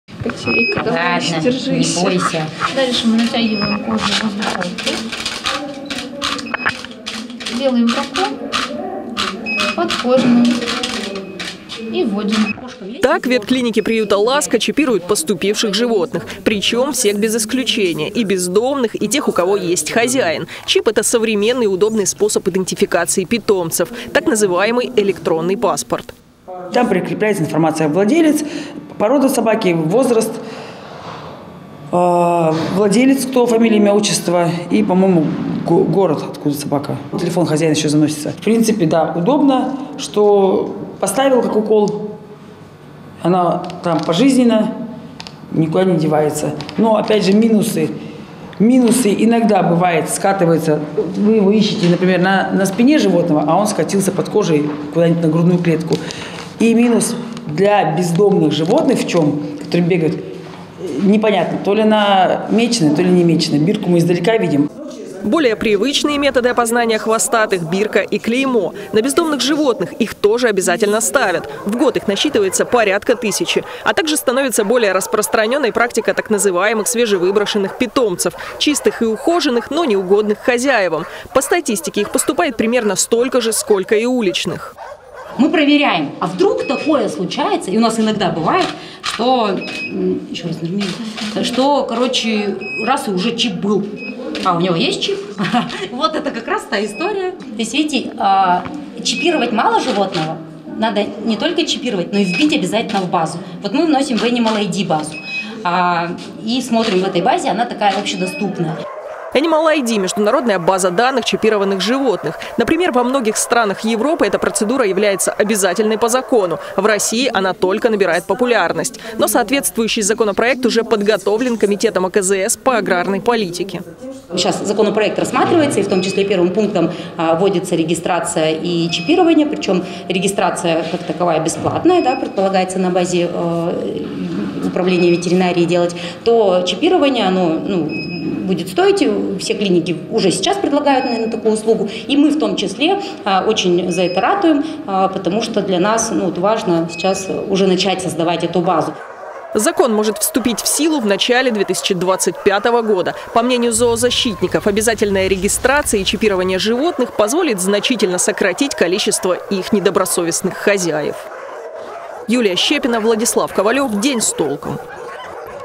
В Барнауле чипируют животных
В Алтайском крае готовится закон об обязательной регистрации и маркировке домашних животных. Чипировать своих питомцев можно уже сейчас.